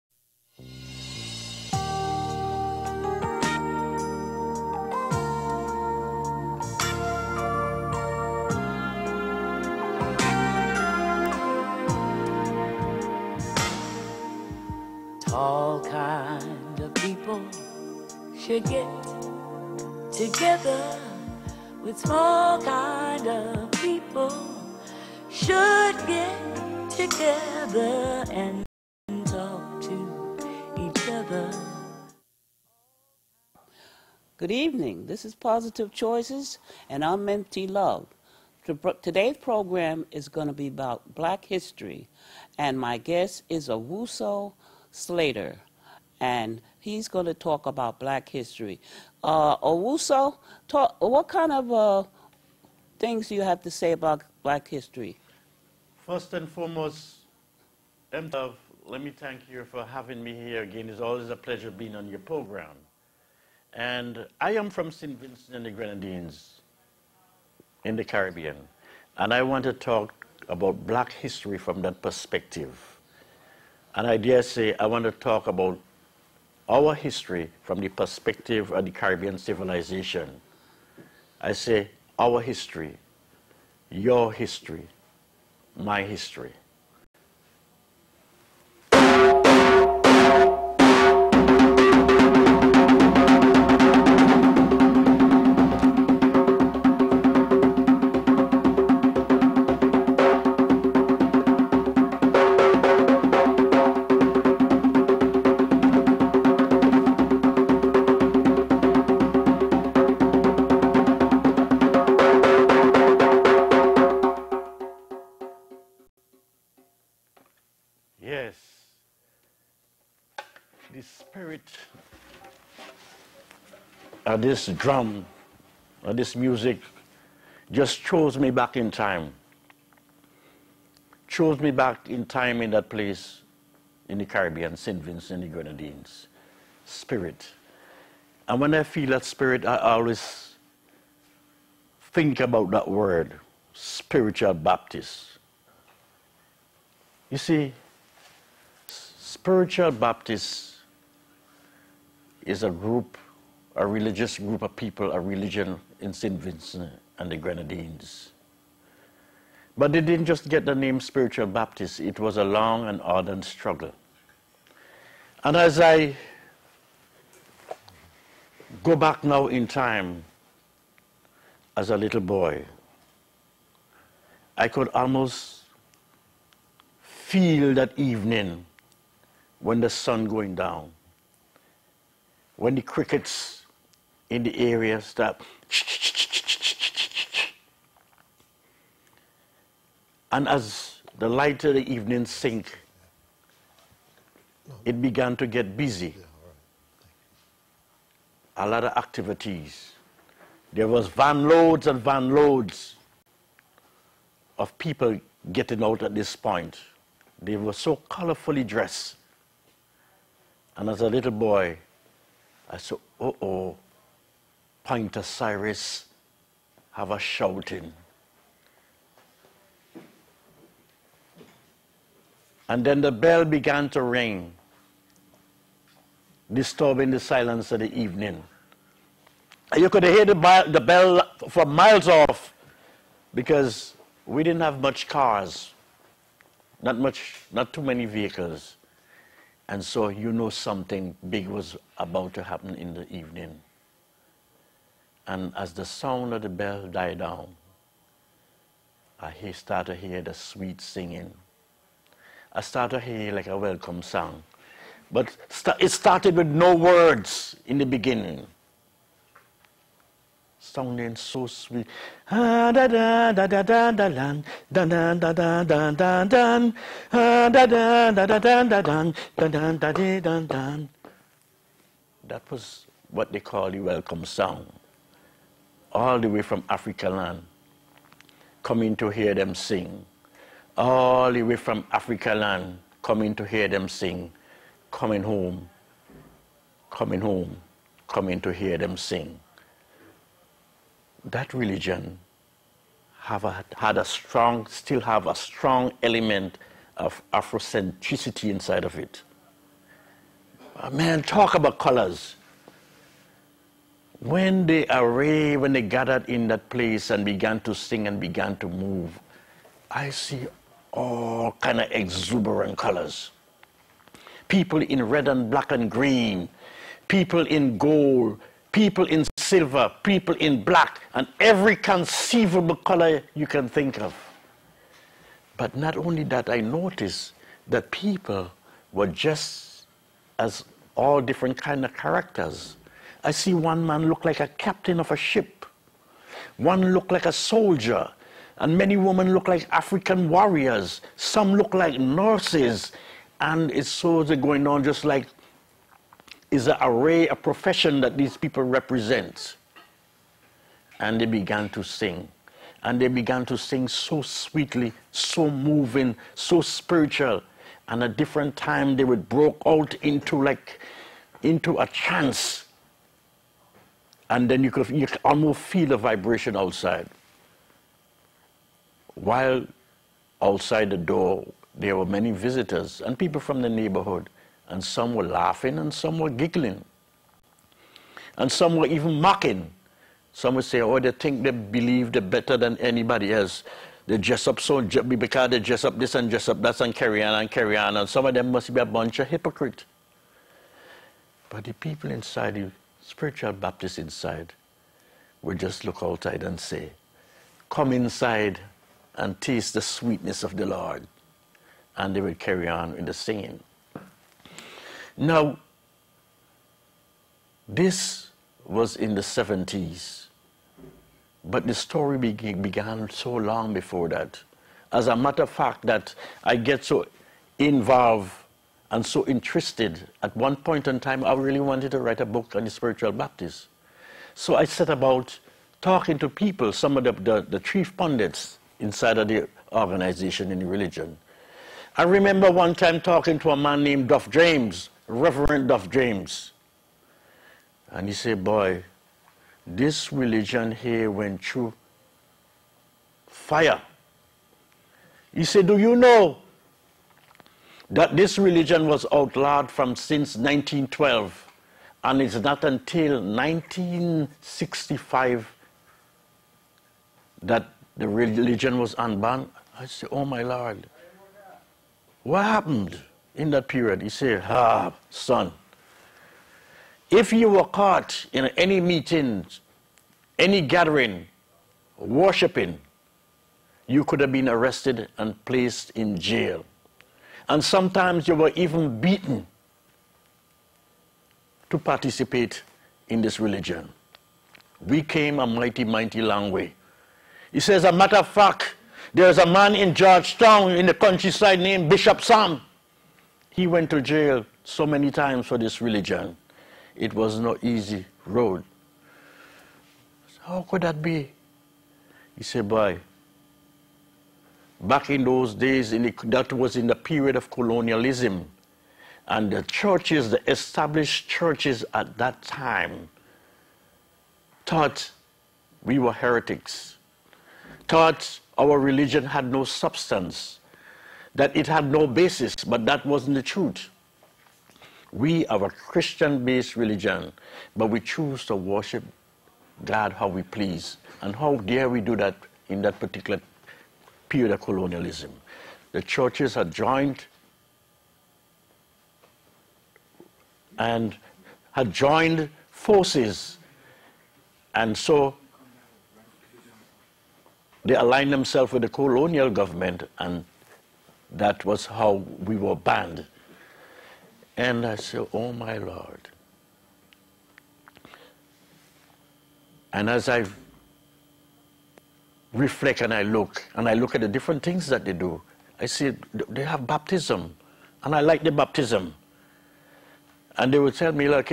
An African-American History Presentation
Owusu Slater is the guest on Positive Choices.